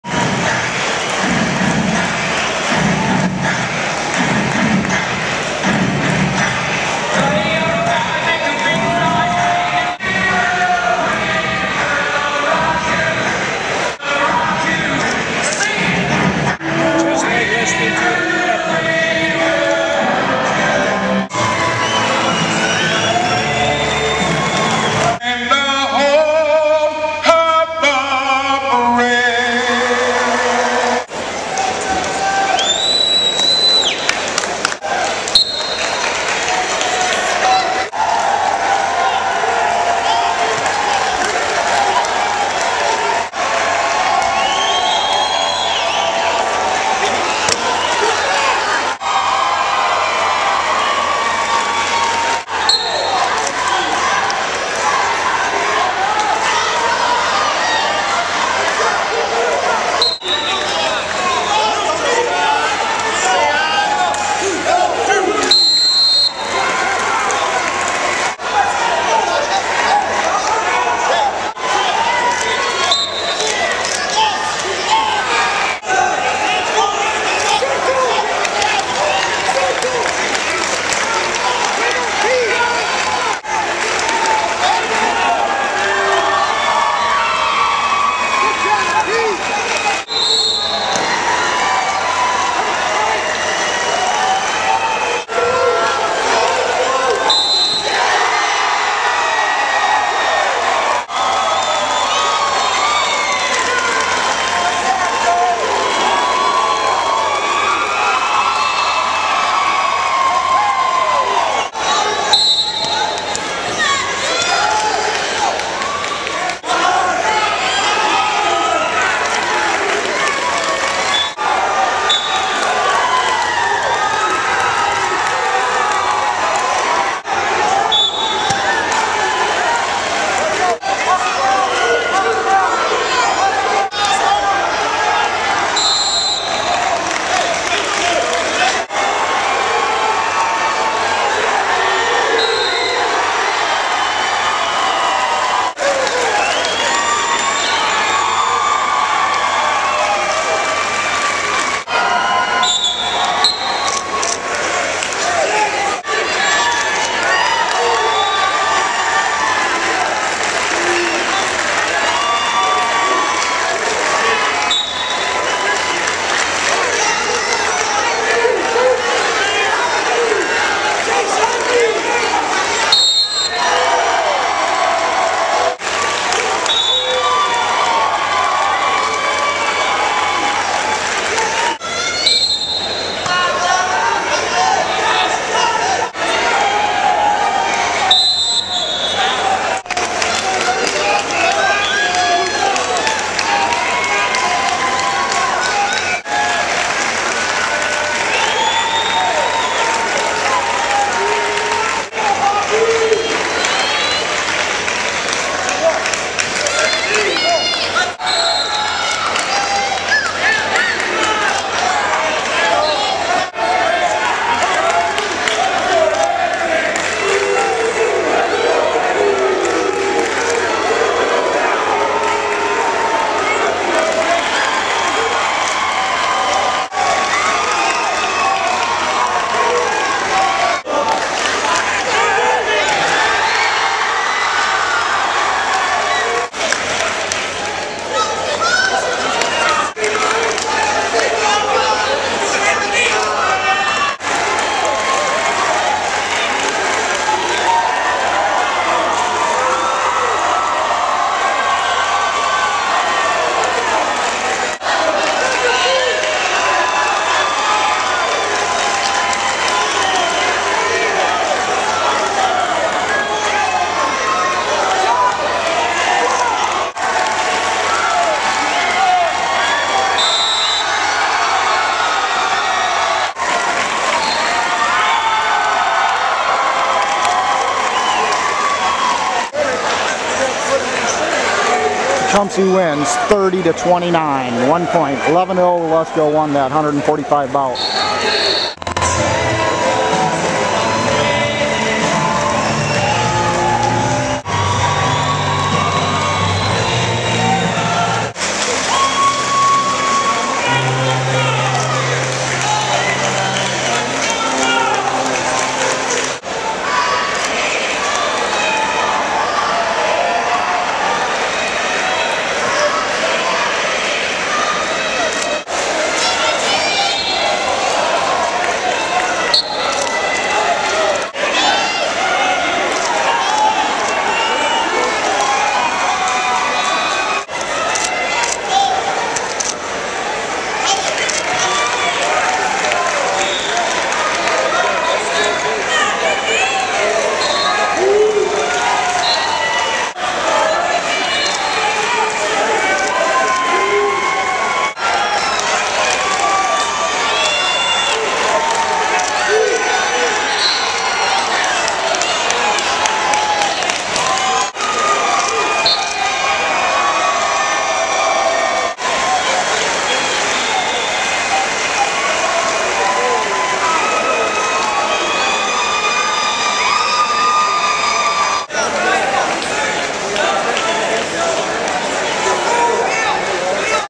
Tecumseh 2012 state wrestling at Battle Creek
Highlights from Tecumseh High School's varsity wrestling team at Division 2, MHSAA state quarterfinals on Friday, Feb. 24, 2012 against Greenville and in the semifinal on Saturday, Feb. 25, in Battle Creek Michigan against defending state champions, St. Johns. Video by the Tecumseh Herald, Mickey Alvarado.